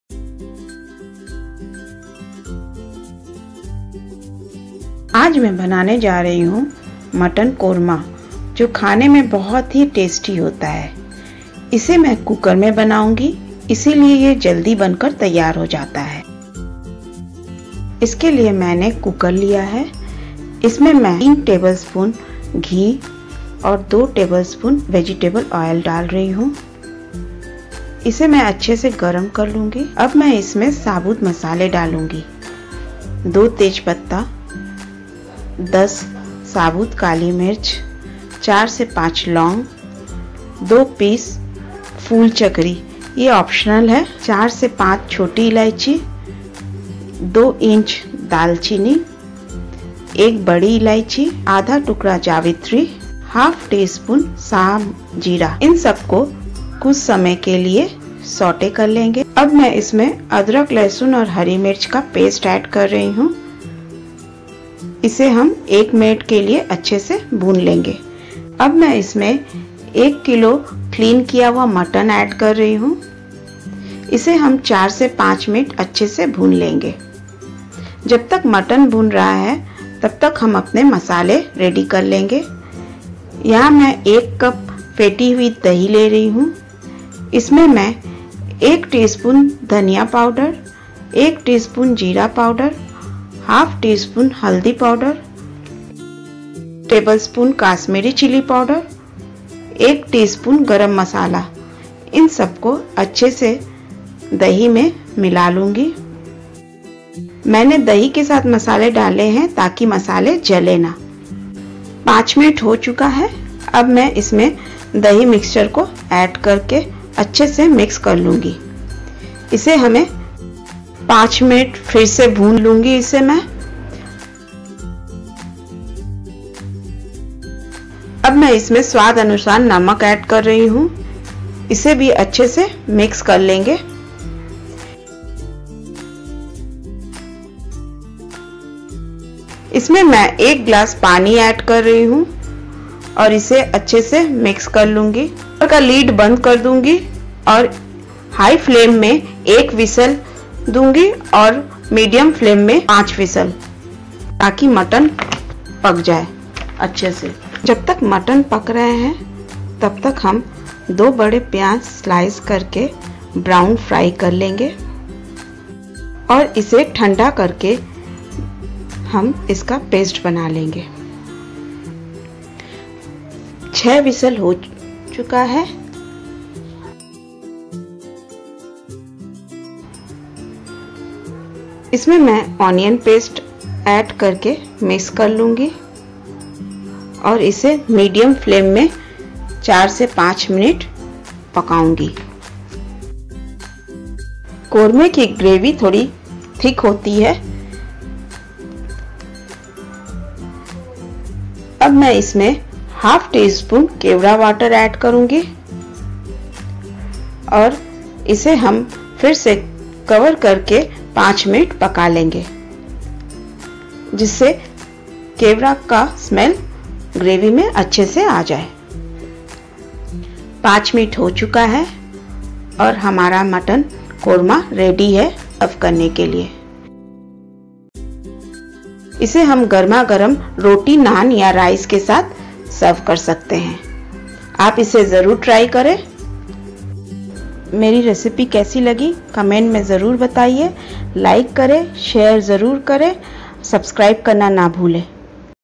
Mutton Korma | Recipe Tailor
below & let me know what you want see next?
time I  upload a video.

INGREDIENTS:
1 kg mutton
3 tbsp ghee
2 tbsp oil
2 bay leaves
2 inch dalchinni
10 shabudh kali mirch
4 laung
2 pc phool chakri(optional)
4-5 pcs chotti ellaichi
1 pc badi ellaichi
1/2 jaivri
1/2 tsp shah jeera
2 large size onion
PASTE- 2 1/2 inch ginger
               6-7 green chillies
               15 garlic cloves
1 cup curd
1 tsp haldi powder
1 tsp dhania powder
1 tsp jeera powder
1 tbsp kashmiri red chilli powder
1 tsp garam masala
salt
1/2 tsp kewra water